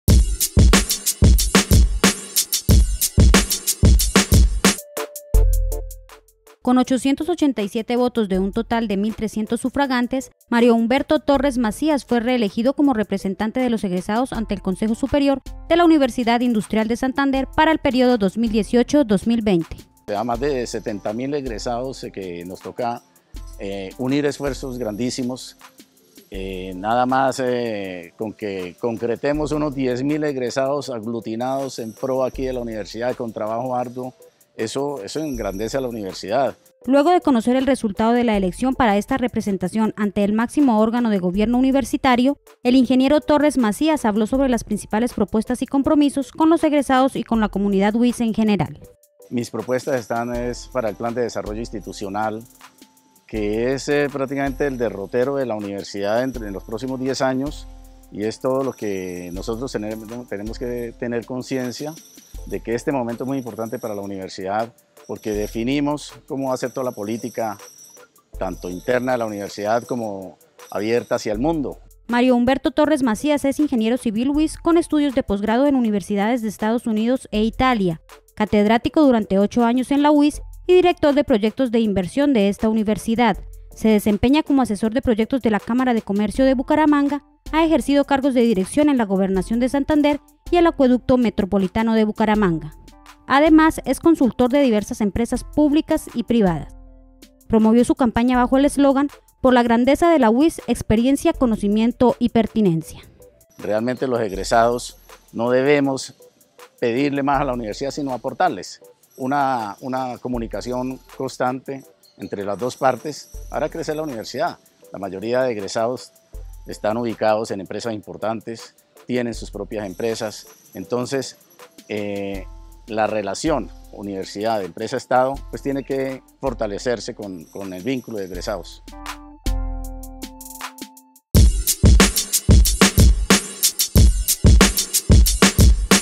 Elegido representante de Egresados ante el Consejo Superior
El ingeniero civil Mario Torres asumirá nuevamente como representante de los Egresados UIS ante el Consejo Superior de la Universidad.